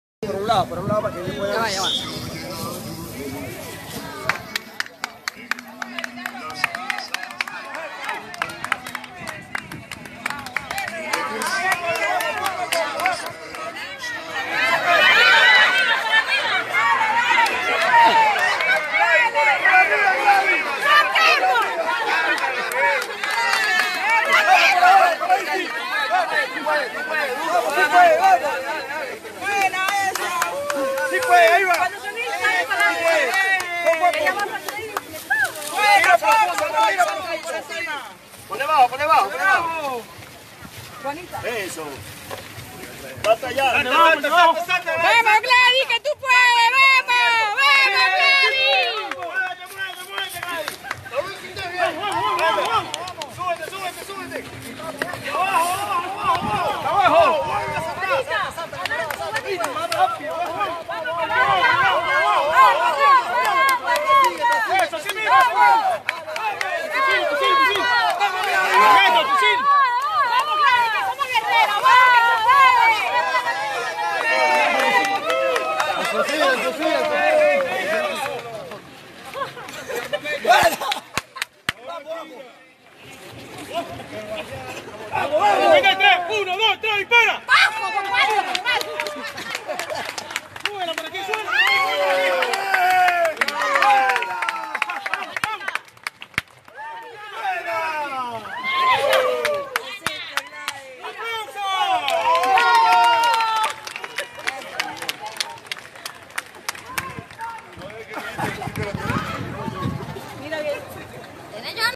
El tremendo entrenamiento de estos milicianos para “defender” Venezuela de una “invasión” gringa
Fuente video: Twitter